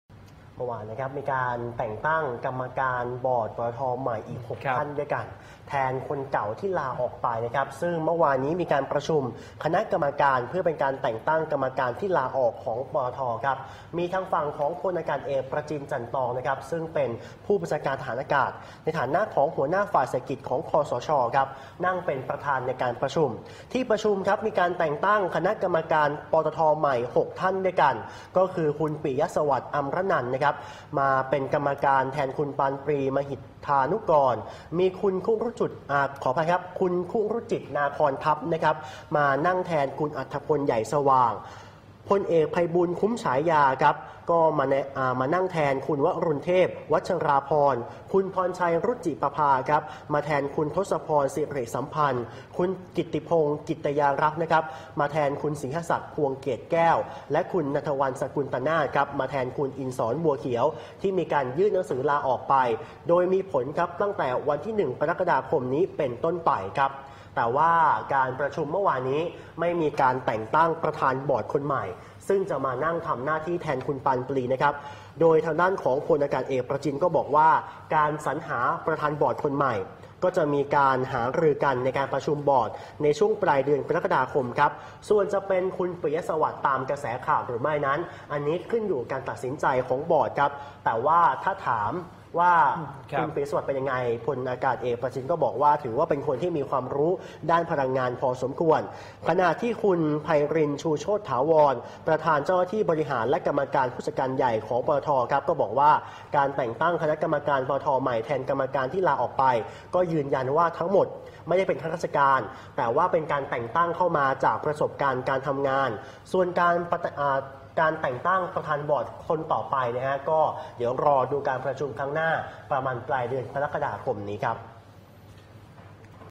ตั้ง 6 บอร์ด ปตท.ใหม่ ปรับทิศทางการทำงาน
Morning news : ตั้ง 6 บอร์ด ปตท.ใหม่ ปรับทิศทางการทำงาน "NOW26" 28-6-57